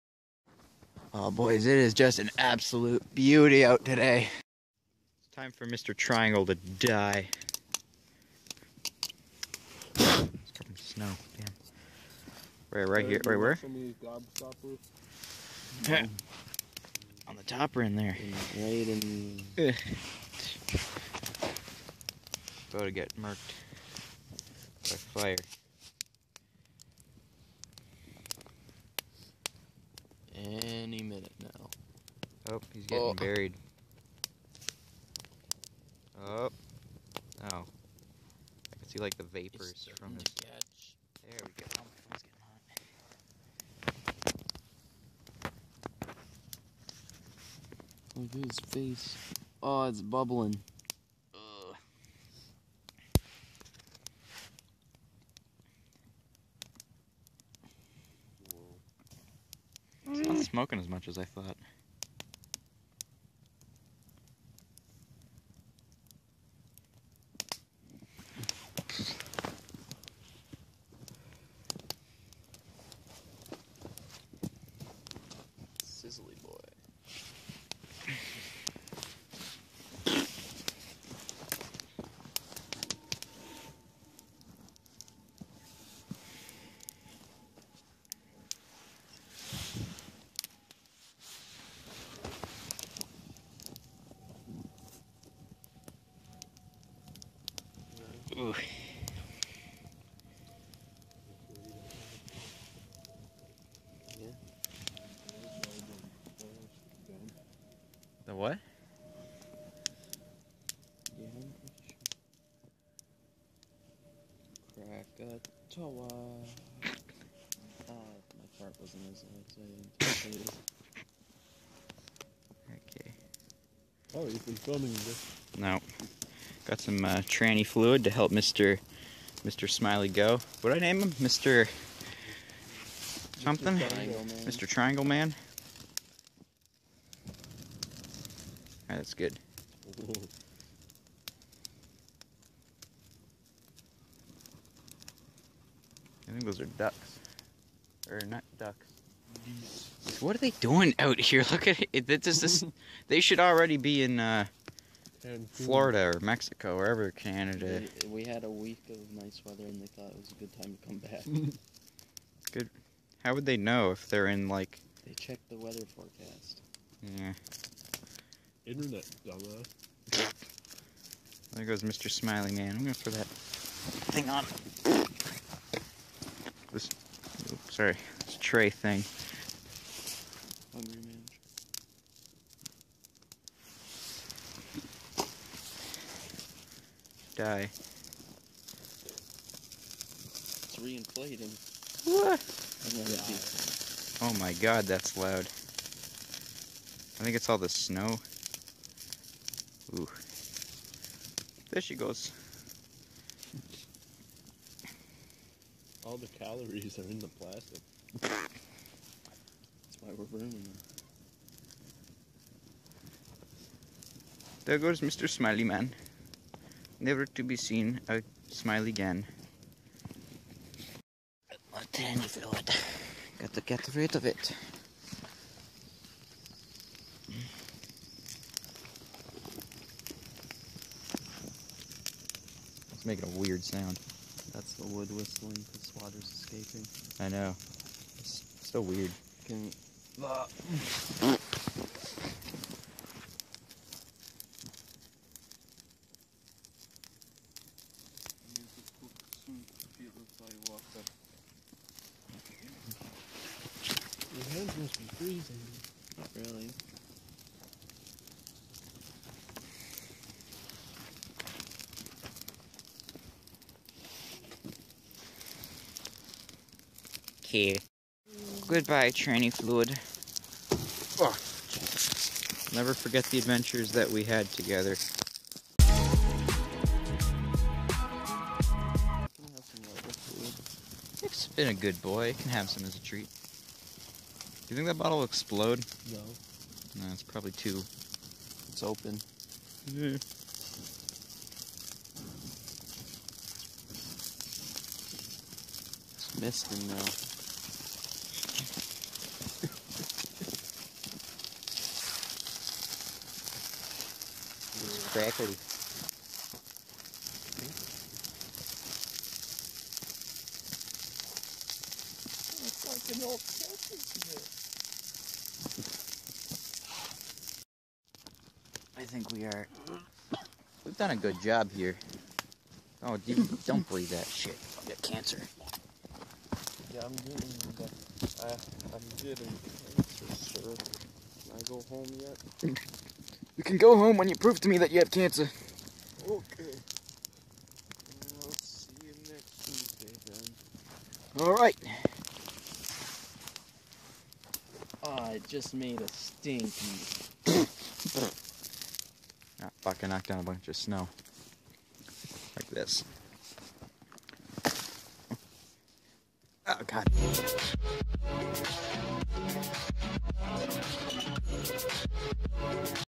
The Burnination Of Mr. Triangle Man
And also a bottle of ATF and lighter fluid and a plastic tray and a fake penny and some other fun stuff.